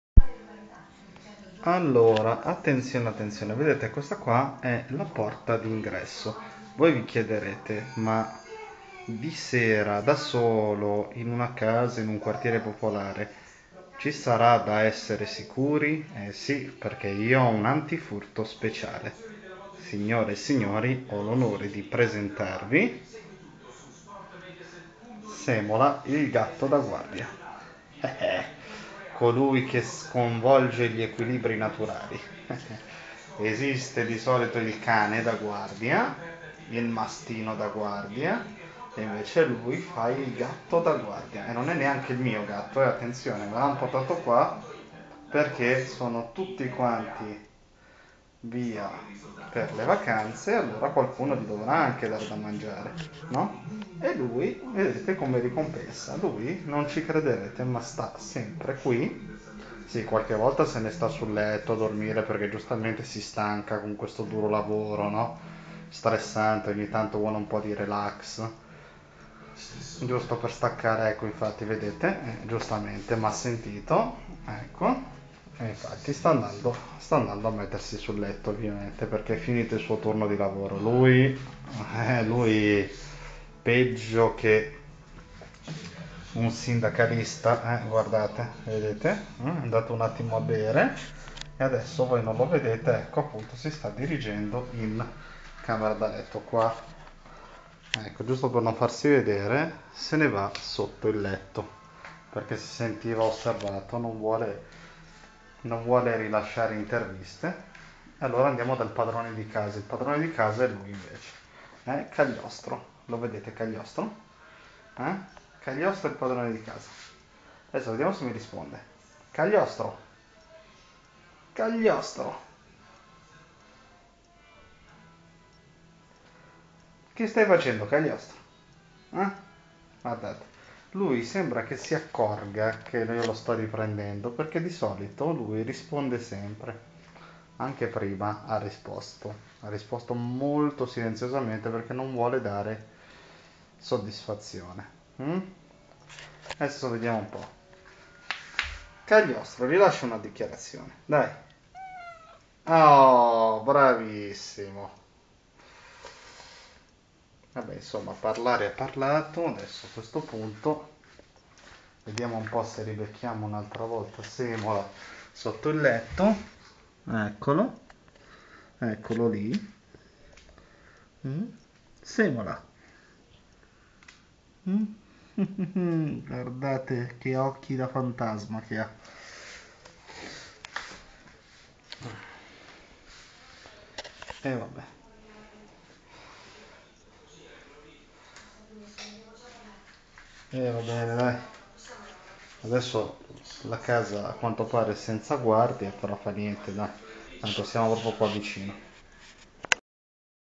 Semola il Gatto da Guardia e Cagliostro il Padrone di casa.
Divertente video di 2 gatti dalle abitudini un po' insolite